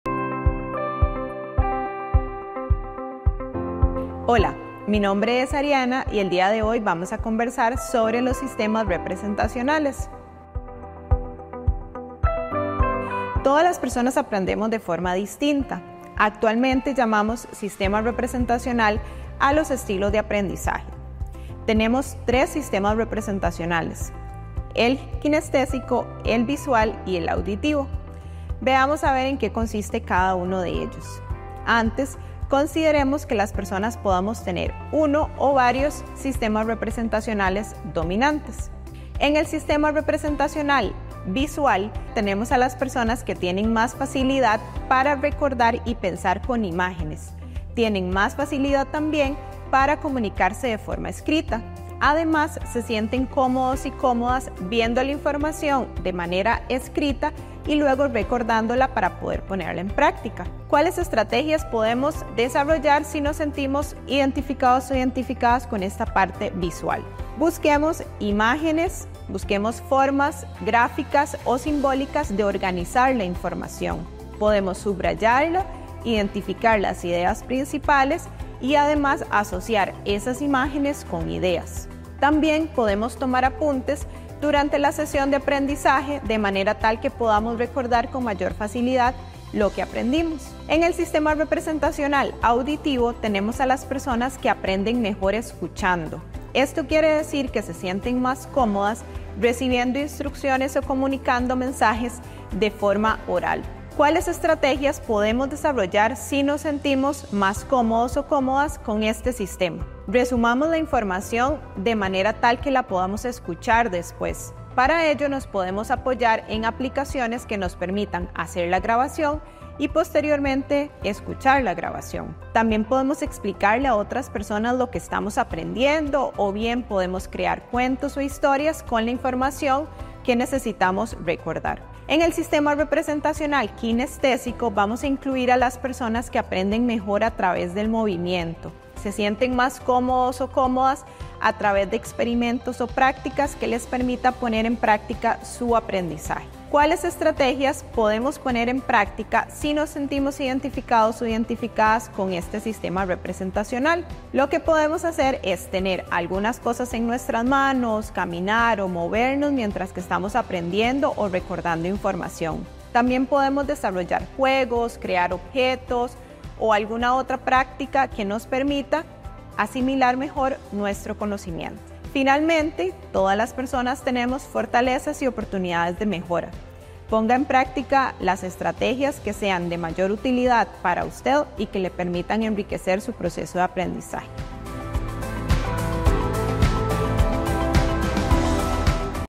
Aprendizaje kinestésico, visual y auditivo ¿Cómo podemos potenciarlo?
¿Sabés que cada persona aprende de forma única? En este video, te compartimos técnicas innovadoras para potenciar tu aprendizaje kinestésico, visual y auditivo.